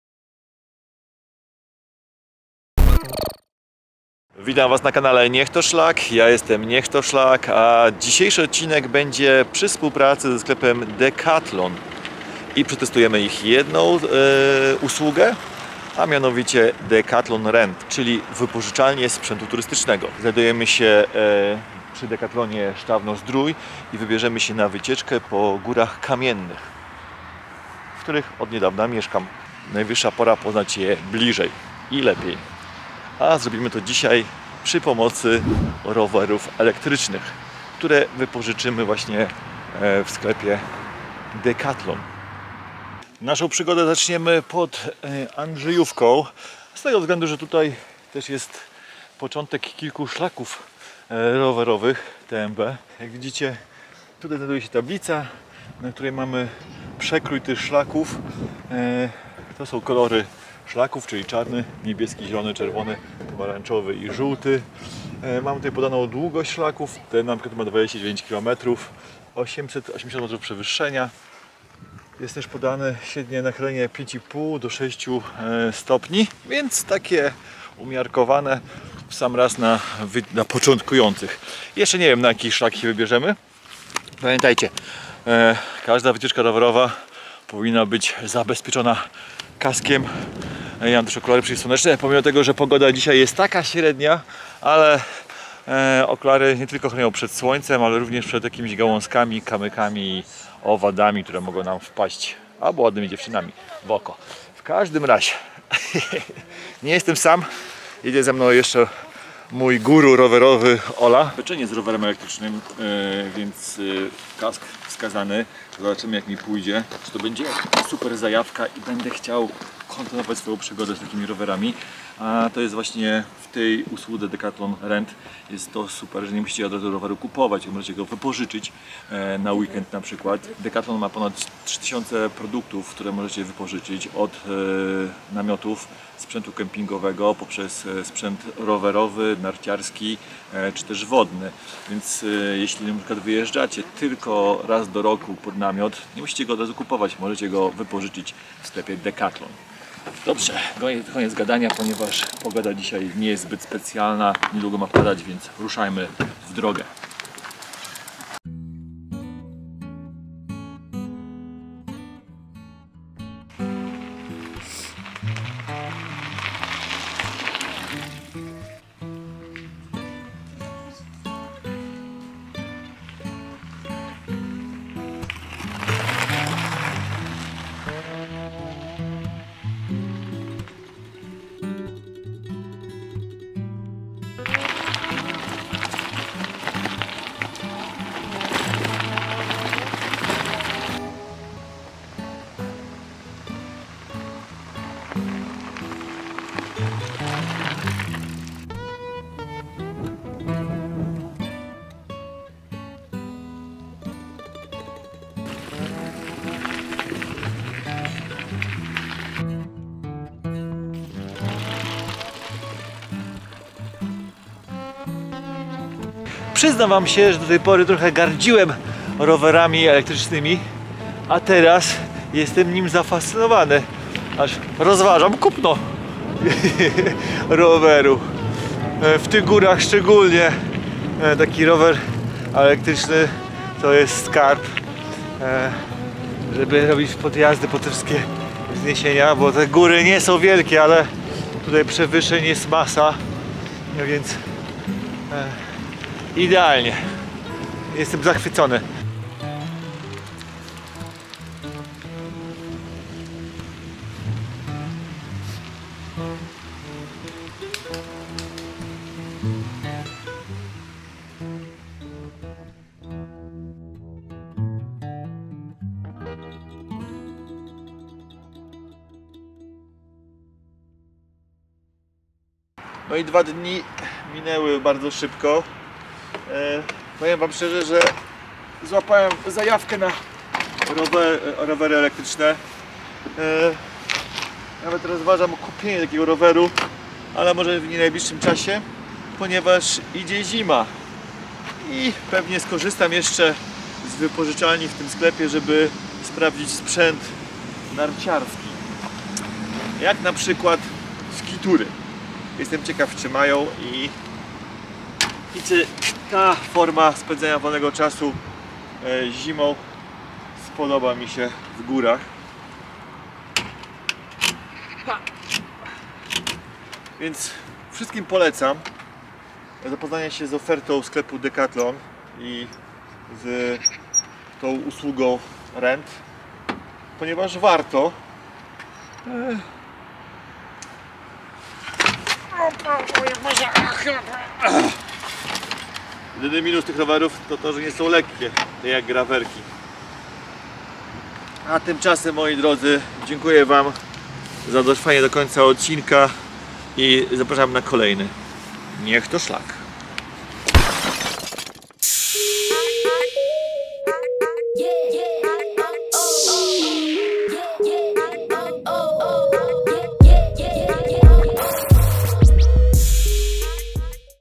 Decathlon Rent: Wypożycz, Eksploruj, Powtarzaj! / NiechToSzlak /decathlon
Cześć wszystkim miłośnikom przygód na świeżym powietrzu! Chce Wam zaprezentować wyjątkową usługę, która zmieni Wasze podejście do turystyki - Decathlon Rent. Dzięki ich platformie możecie wypożyczyć sprzęt turystyczny, który sprawi, że Wasze wyprawy staną się jeszcze bardziej spektakularne!

Decathlon Rent to nie tylko możliwość wynajmu sprzętu na terenie całej Polski, to także szansa na odkrywanie nowych pasji i aktywności. Ich szeroki katalog produktów zawiera wszystko, czego potrzebujecie, by cieszyć się różnorodnymi formami aktywności outdoorowej, bez konieczności ogromnych wydatków na zakup sprzętu.

Dlaczego zatem zamykać się na jedną dziedzinę, skoro możecie próbować czegoś nowego za każdym razem? Odkryjcie swoje nowe pasje i cieszcie się turystycznymi wyprawami bez ograniczeń!"